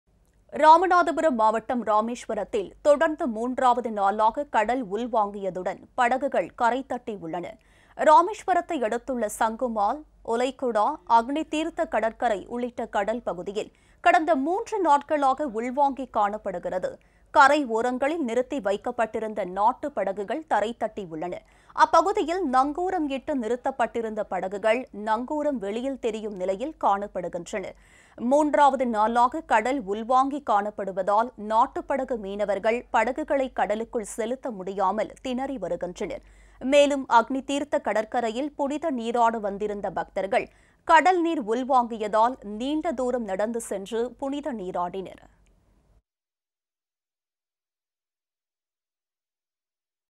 தொடர்ந்து 3-வது நாளாக உள்வாங்கிய அக்‍னி தீர்த்த கடல் | Rameshwaram | Sea Water
Rameshwaram | SeaWater | தொடர்ந்து 3-வது நாளாக உள்வாங்கிய ராமேஸ்வரம் அக்‍னி தீர்த்த கடல்